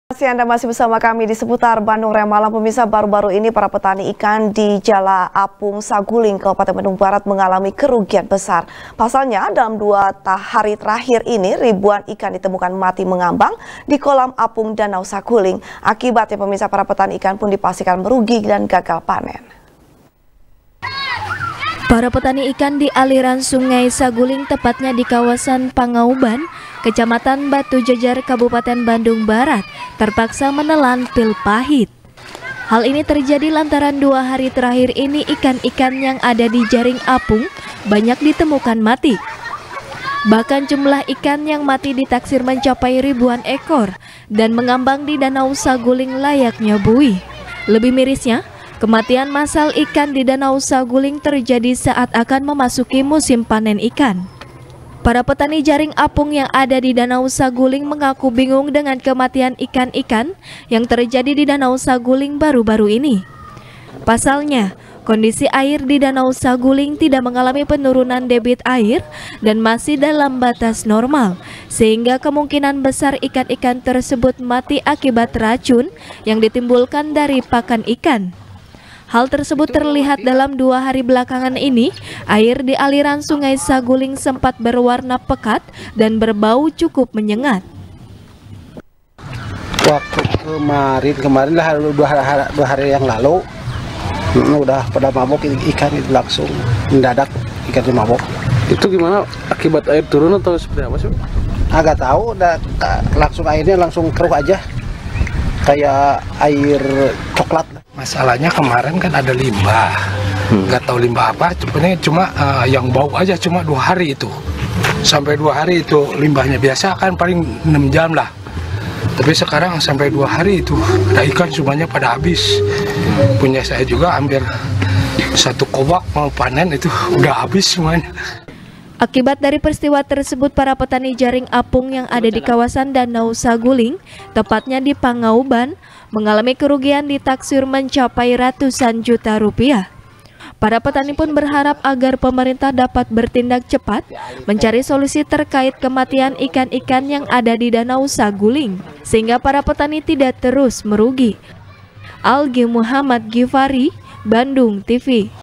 Ribuan Ikan Mati, Petani Ikan Jaring Apung Di Danau Saguling Gagal Panen | SBR | BANDUNGTV
SBR | BANDUNGTV | FARHAT MUMTAZ | Baru-Baru Ini, Para Petani Ikan Di Jala Apung Saguling, Kabupaten Bandung Barat, Mengalami Kerugian Besar. Pasalnya Dalam Dua Hari Terakhir Ini, Ribuan Ikan Ditemukan Mati Mengambang, Di Kolam  Apung Danau Saguling. Akibatnya, Para Petani Ikan Pun Dipastikan Merugi, Dan Gagal Panen.